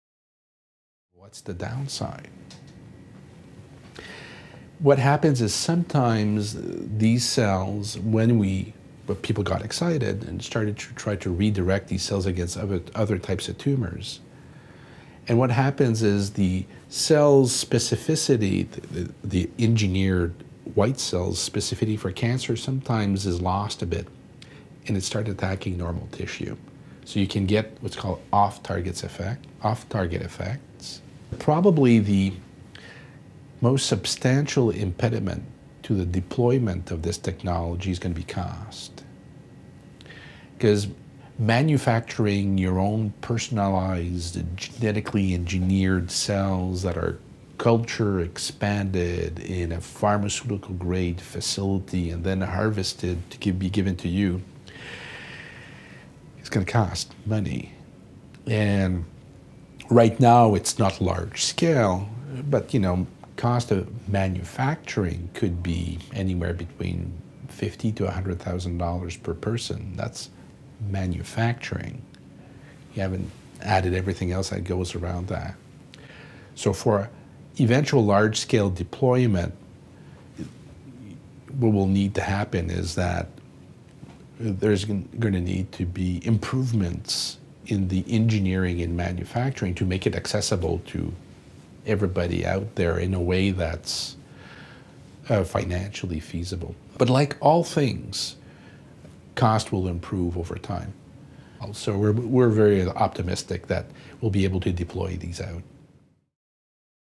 Dr. Jacques Galipeau: Immunotherapy and Cancer- Limitations of Immunotherapy
Dr. Jacques Galipeau of the Winship Cancer Institute of Emory University discusses some of the impediments to using immunotherapy in the treatment of cancer.

13